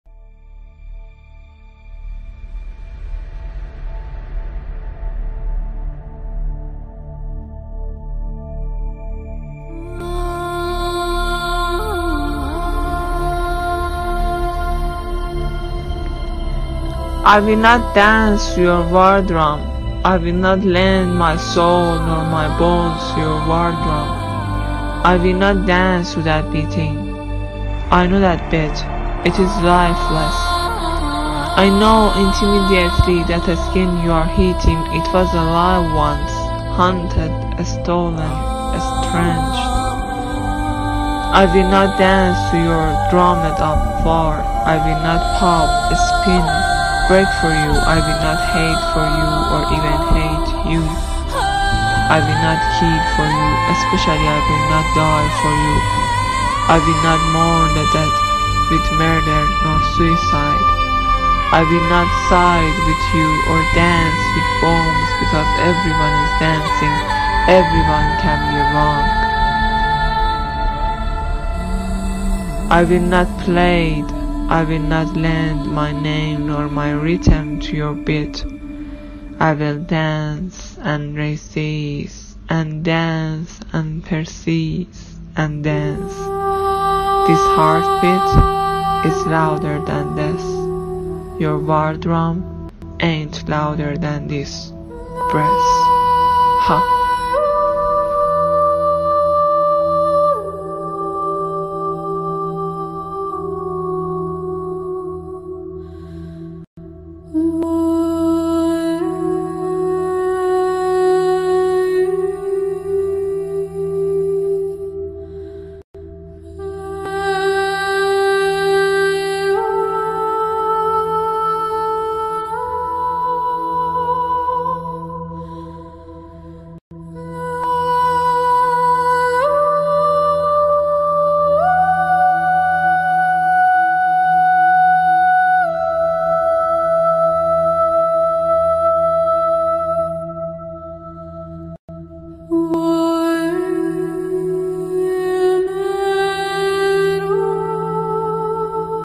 Woman from Iran ( Zardeh )/Bird of Hope
This is the photo of a woman in Zardeh ( A village in Iran )
A lot of people are victims of chemical weapons in this village but all I see in their woman faces is hope and courage for life and I think this is the time that war is became Humiliation.
What I Will

Photo by : David Verberckt
Video by : Behnaz Monfared
Poem by: Suheir Hammad

dance to your war
drum. I will
not lend my soul nor
my bones to your war
drum. I will
not dance to your
beating. I know that beat.
It is lifeless. I know
intimately that skin
you are hitting. It
was alive once
hunted stolen
stretched. I will
not dance to your drummed
up war. I will not pop
spin break for you. I
will not hate for you or
even hate you. I will
not kill for you. Especially
I will not die
for you. I will not mourn
the dead with murder nor
suicide. I will not side
with you nor dance to bombs
because everyone else is
dancing. Everyone can be
wrong. Life is a right not
collateral or casual. I
will not forget where
I come from. I
will craft my own drum. Gather my beloved
near and our chanting
will be dancing. Our
humming will be drumming. I
will not be played. I
will not lend my name
nor my rhythm to your
beat. I will dance
and resist and dance and
persist and dance. This heartbeat is louder than
death. Your war drum ain’t
louder than this breath.